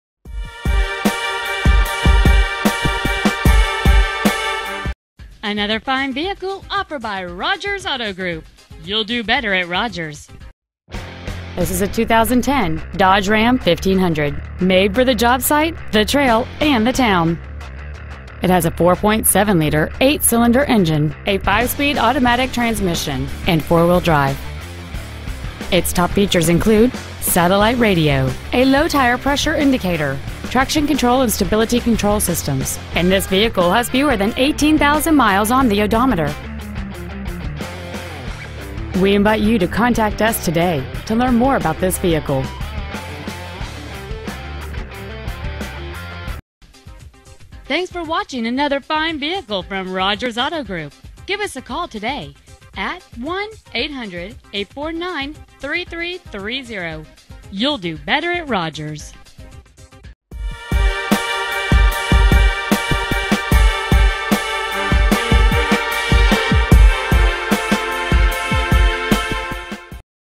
2010 Dodge Ram 1500 Shelby NC
We've been honored to serve the Shelby NC area, we promise that your experience at our dealership will exceed your expectations!

 Year: 2010
 Make: Dodge
 Model: Ram 1500
 Engine: Gas/Ethanol V8 4.7L/287
 Trans.: Automatic
 Exterior: Bright Silver Metallic
 Miles: 17,117
 Interior: Dark Slate Gray

 4421 East Dixon Blvd.

 2010 Dodge Ram 1500 Shelby NC